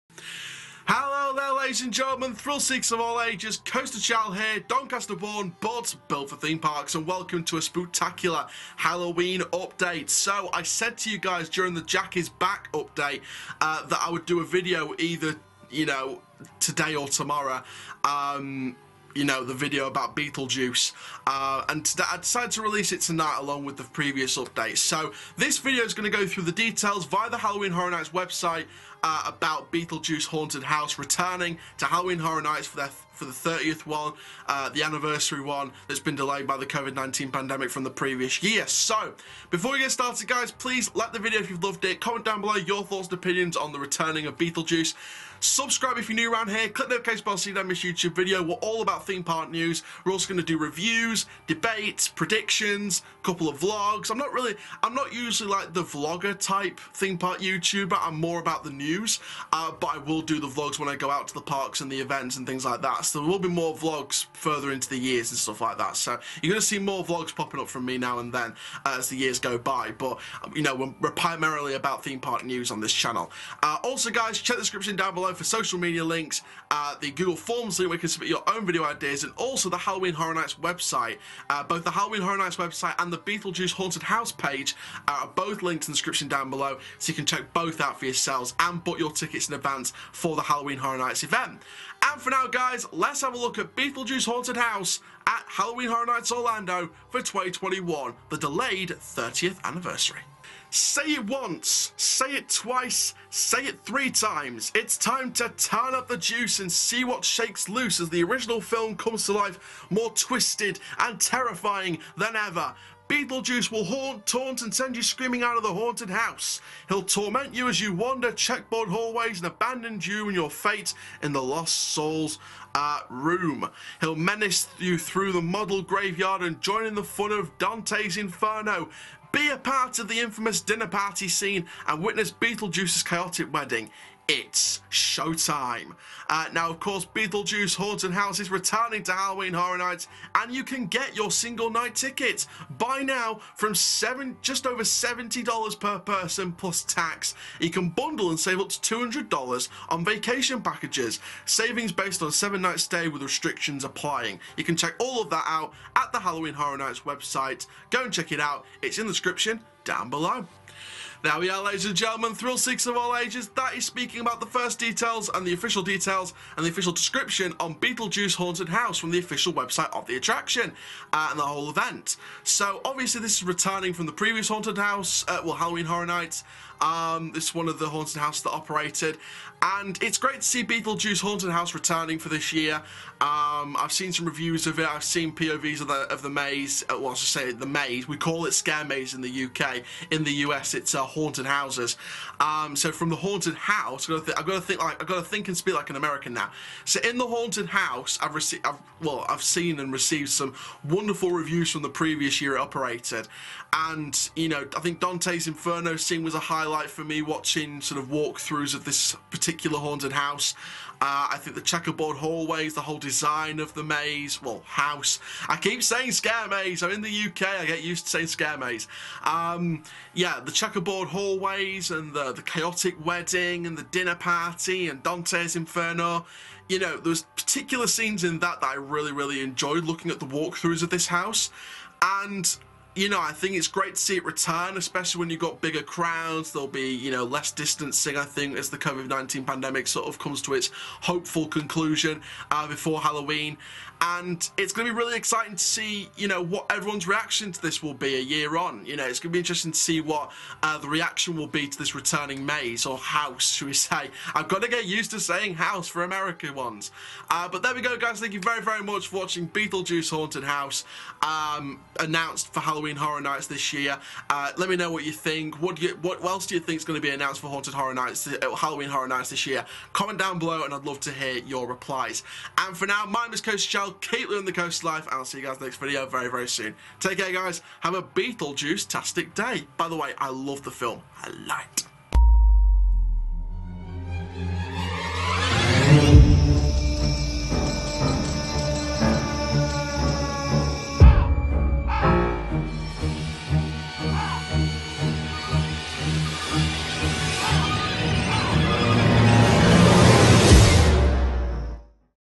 Universal Studios Orlando Has Confirmed Beetlejuice Will Return For Halloween Horror Nights 2021
On today's Halloween Update, I'll be sharing the official details on the first official haunted house for the 30th anniversary Halloween Horror Nights event at Universal Studios Orlando - Beetlejuice Haunted House!

Intro: 0:00
Facts and Stats: 1:40
My Thoughts: 2:40
Outro: 4:42